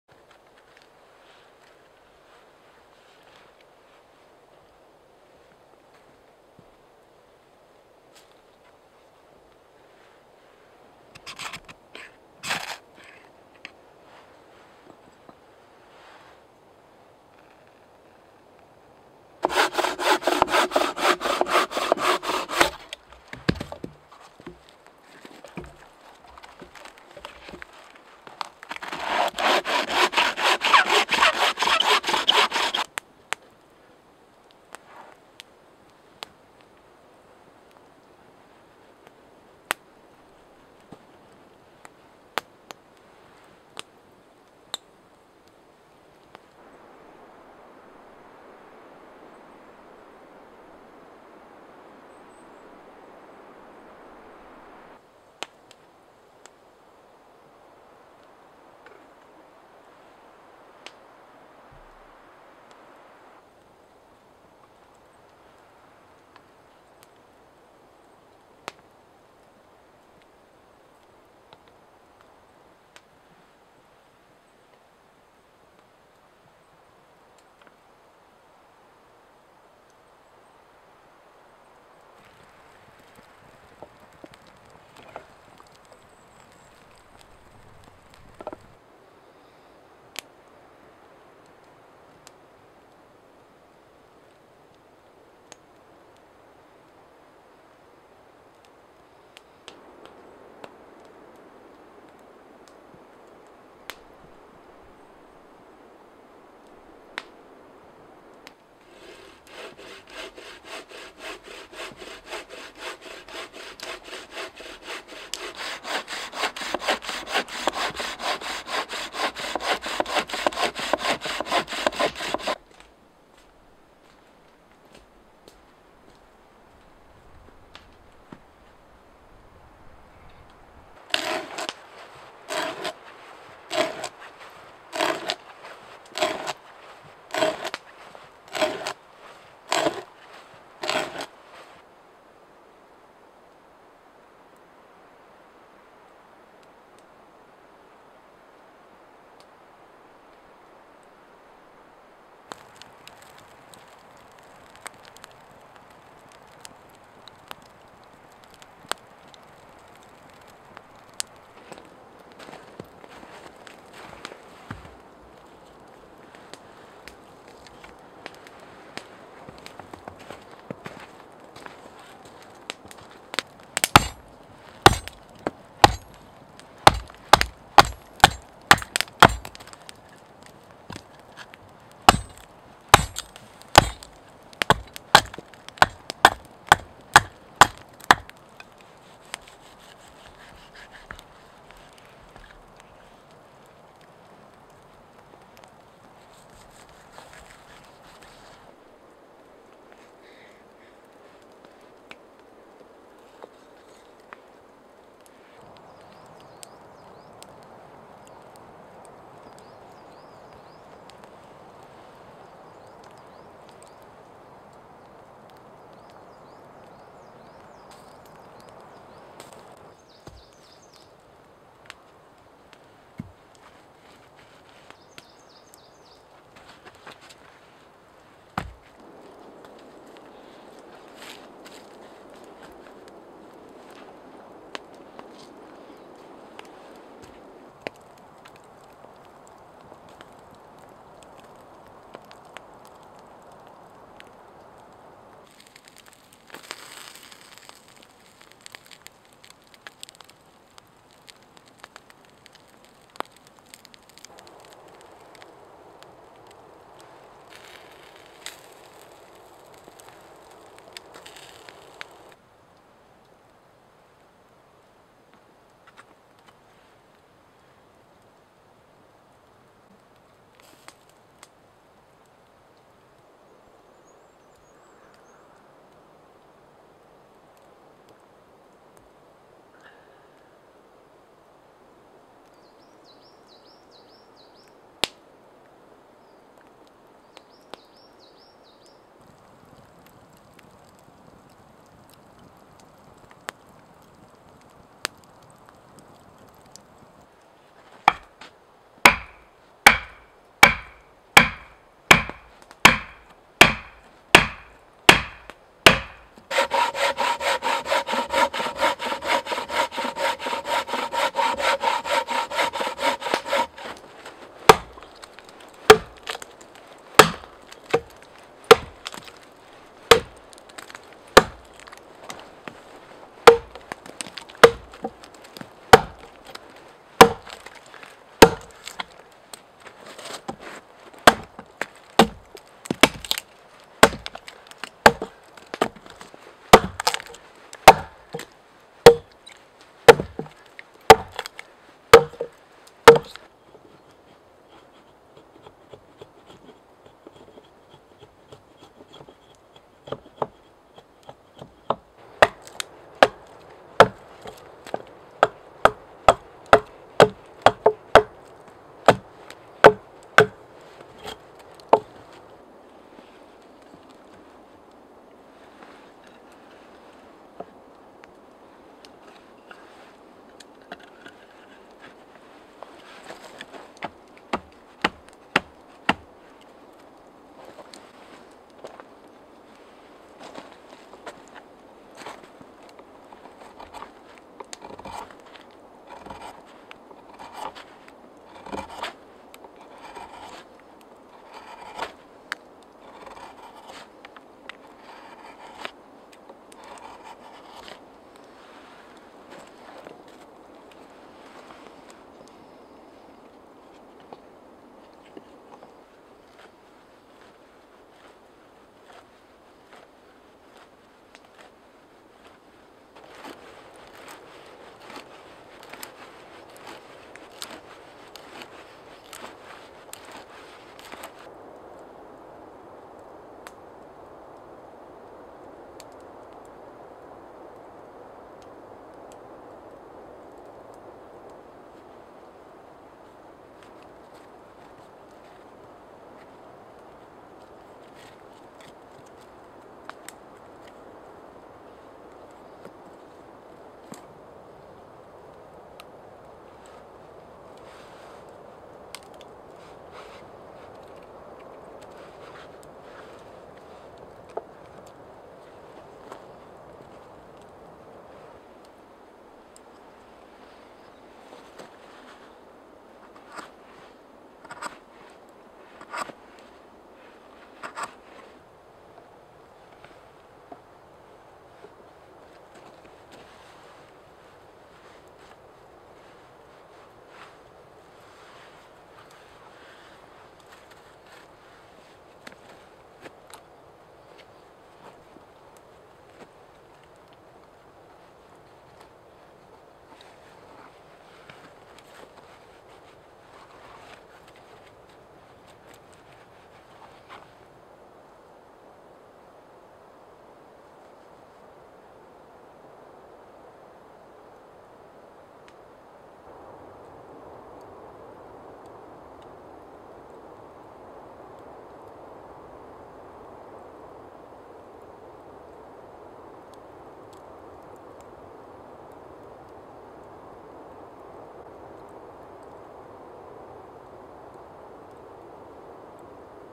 Bushcraft trip - making carving bench - permanent a-frame camp series [part 3 - short version]
I am enjoying an overnight in my permanent bushcraft camp here in Denmark. Making some great meat and a carving bench, improving the fire place and a few other things - and just enjoying the camp site. I get snow fall on day one. I will use the carving bench for making bowls and spoons in future videos.

Location: Denmark - previous viking country. West coast of Jutland - sand, big sea, spruce and pine forrest. Some of the most remote in Denmark, but a lot of people in Denmark so limited how remote it can get. A lot of birds and deer. No bears only a few wolves - nothing dangerous for campers. If you want to do the same in Denmark do you need to be the owner or get the owners permission.

Consuming: Water, pine/spruce tea, chaga tea. Flour and unknown meat balls. Cow.

Some of the gear used in the video:
1. Sweater = unknown.
2. Pot and kettle = made by "eagle" 1.5 and 10 liters.
3. Pants = made by "Ridgeline"
4. Axe = made by "Kellokoski" model 18
5. Carving tools = mora 106, Pfeil 8L 25mm and 7A 25mm
6. Boots = made by "Redback" original boots
7. Tent = polish puncho army lavvu
8. Backpack = made by "Fjallraven" Kajka 65L
9. Sharpening stone = made by "Gransfors Bruks"
10. Jacket = Lundhags Sprek

Video gear: Nikon D7000, Nikon 50 1.8, Røde videomacro, iMovie.